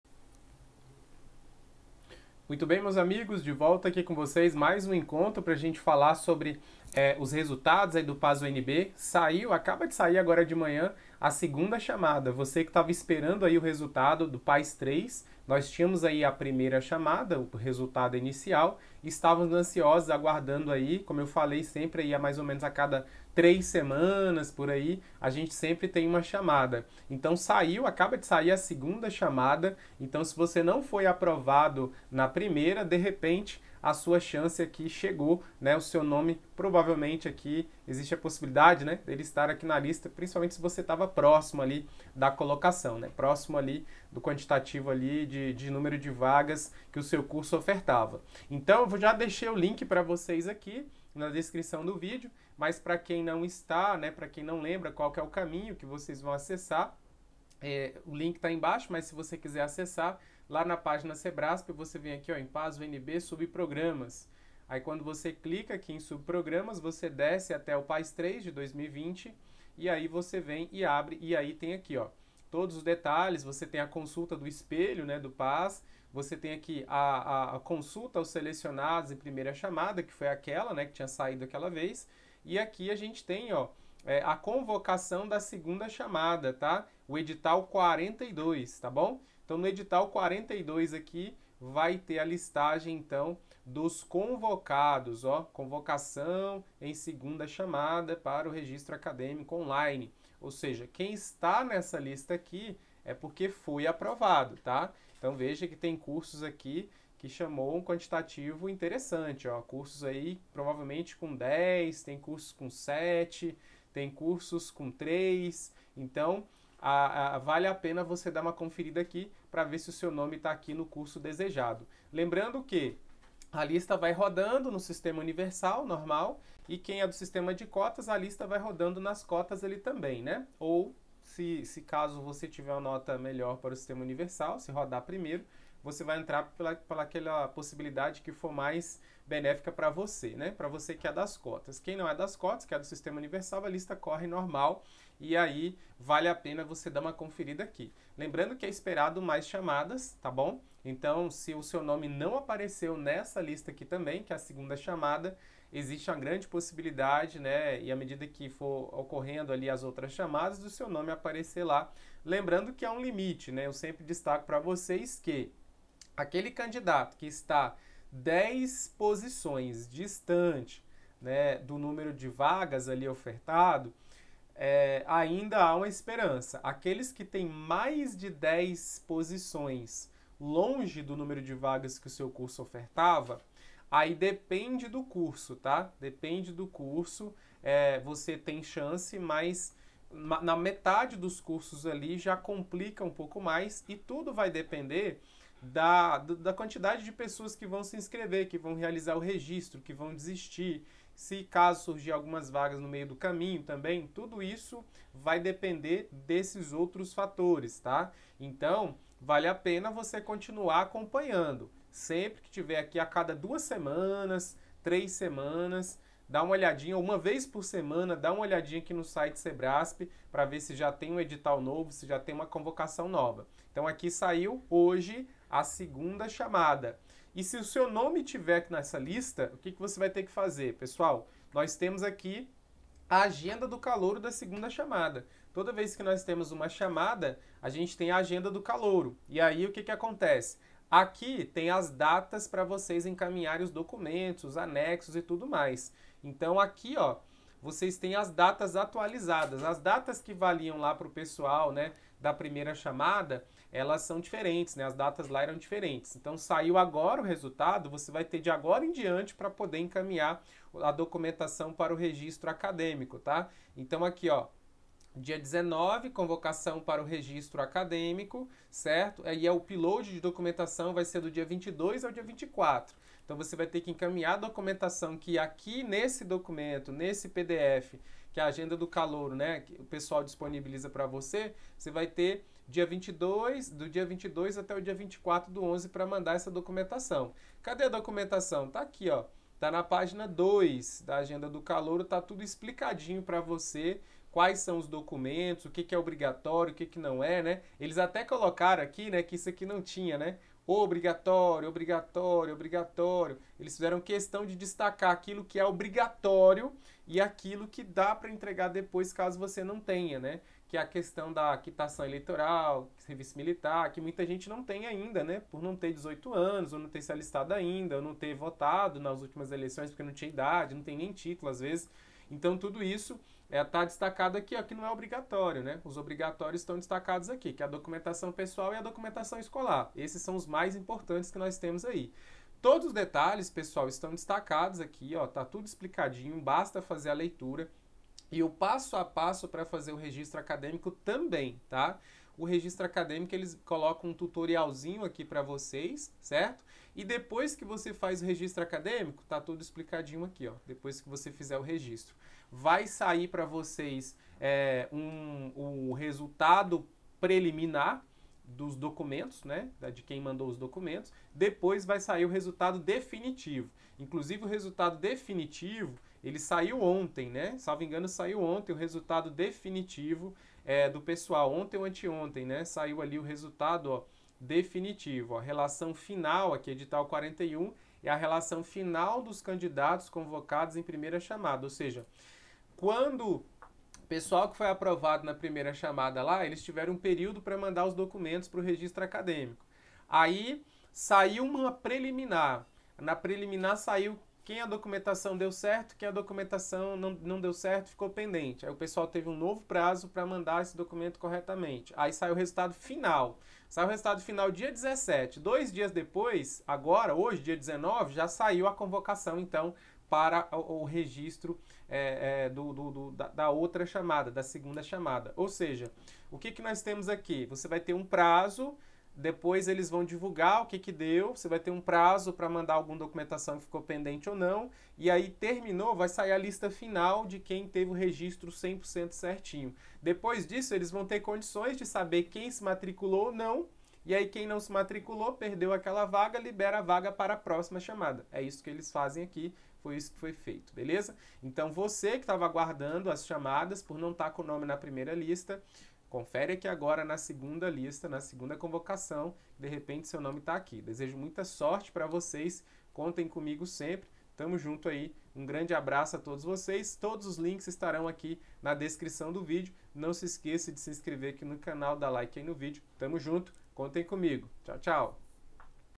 URGENTE!!! SAIU A SEGUNDA CHAMADA DOS APROVADOS | PAS UNB 2020
Saiu a segunda convocação para os aprovados no PAS UNB 2020.

Link para a lista dos aprovados em segunda chamada